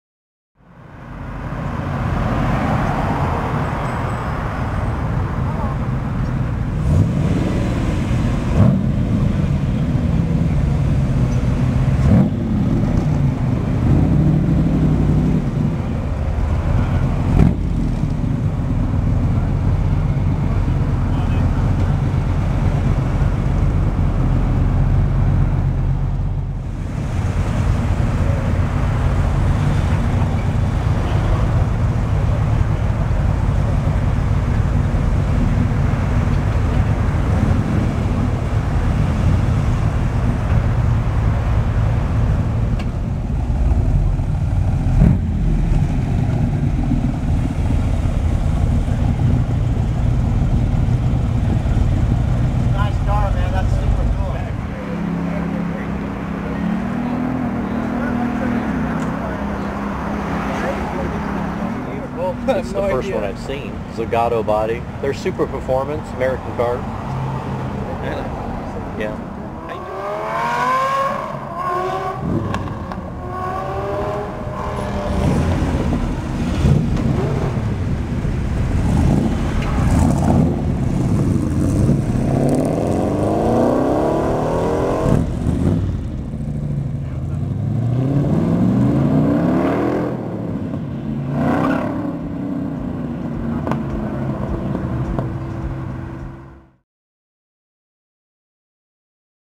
AC 378 GT Zagato
A very rare AC 378 GT Zagato arriving at and leaving Lamborghini Newport Beach's Supercar Show.

(June 1, 2013 / Costa Mesa, CA)

Also caught on this video: Superformance Cobra